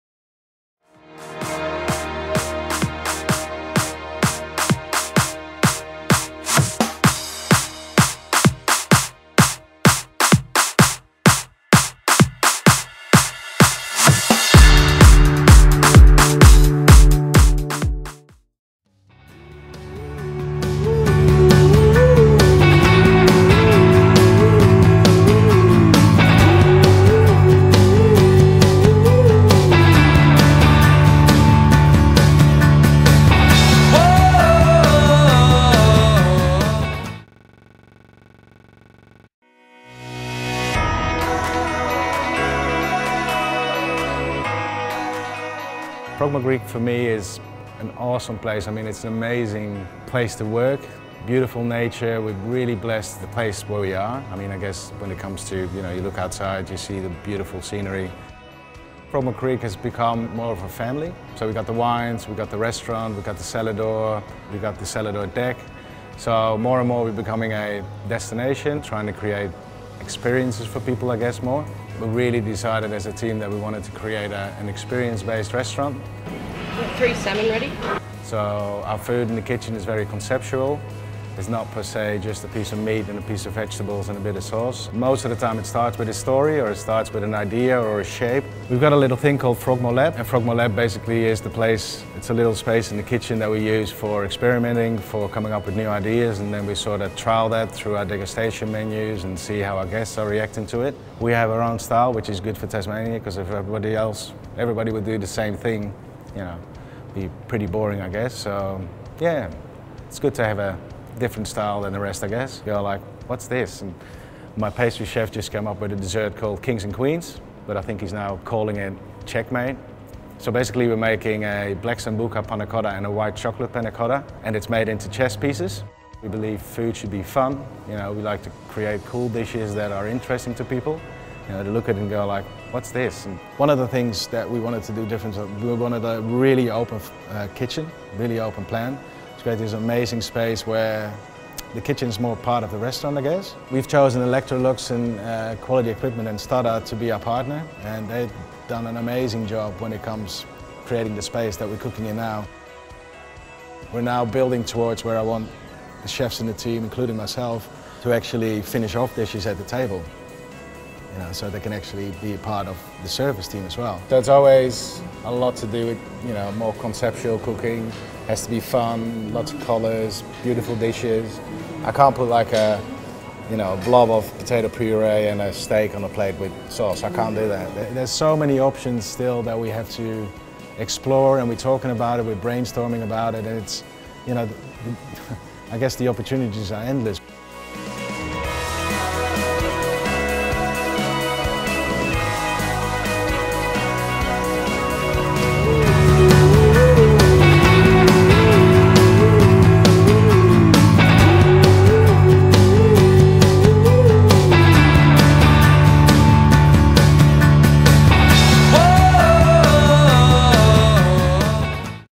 Frogmore Creek | Stoddart
A unique look inside the  Restaurant at Frogmore Creek in Tasmania, provided by Electrolux Professional and Stoddart.